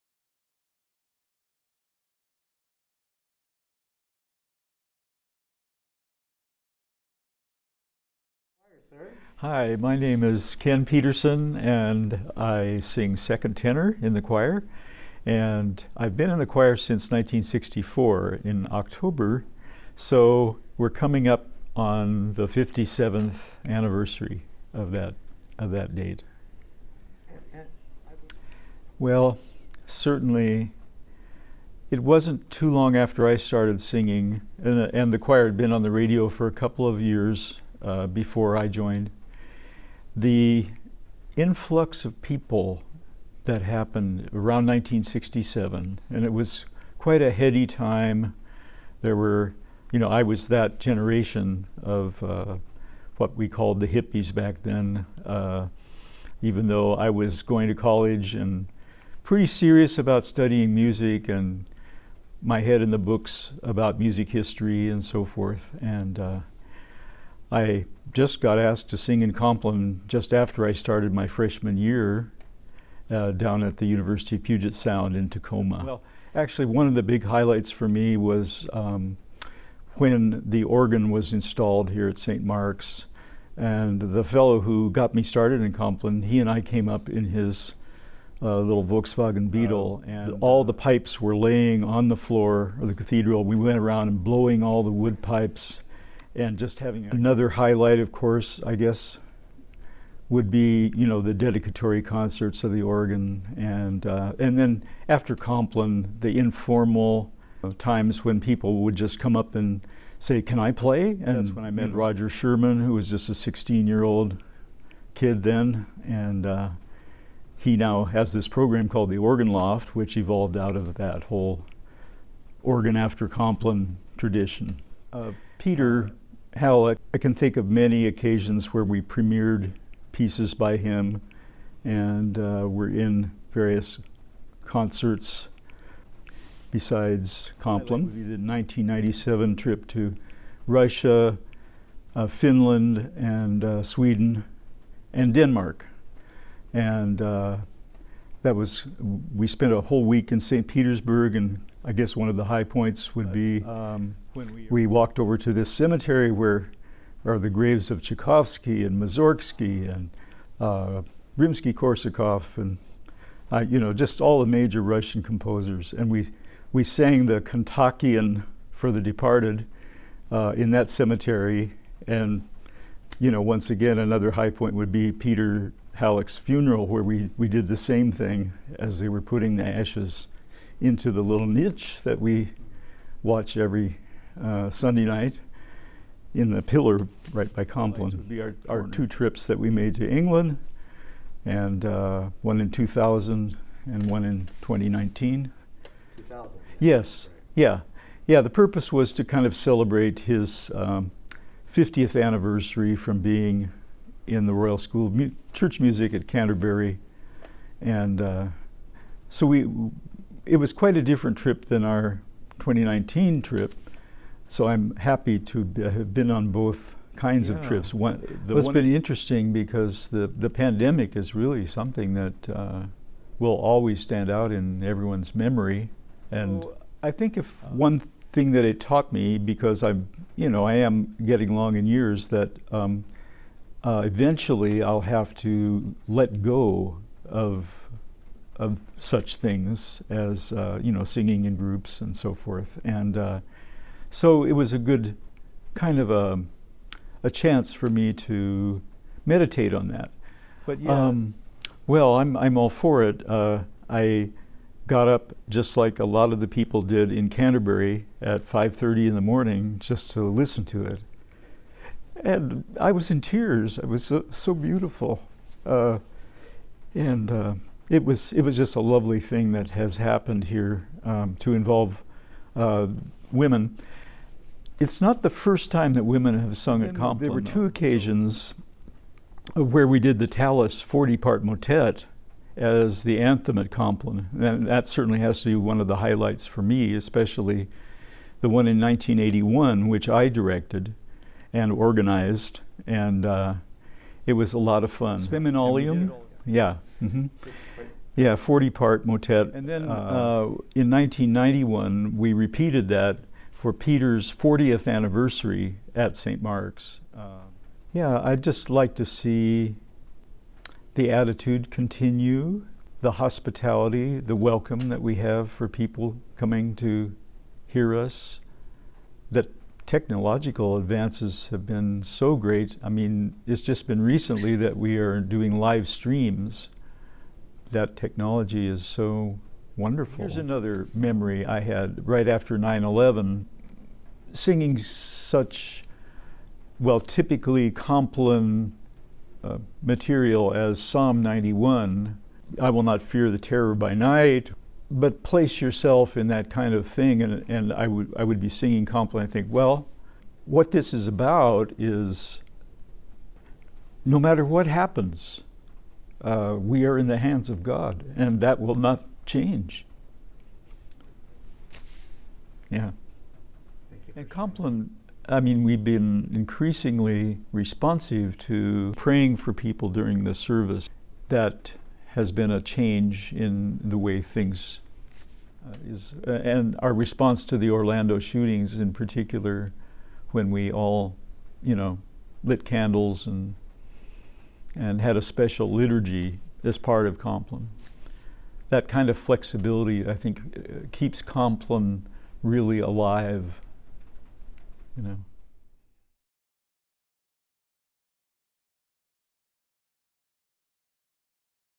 65th Anniversary Compline Choir Interview feat. Ken Peterson
The Compline Choir celebrates 65 years of singing the office of Compline at St. Mark's Episcopal Cathedral in Seattle and an interview with member Ken Peterson as he reflects on a remarkable 57 years of singing in the choir.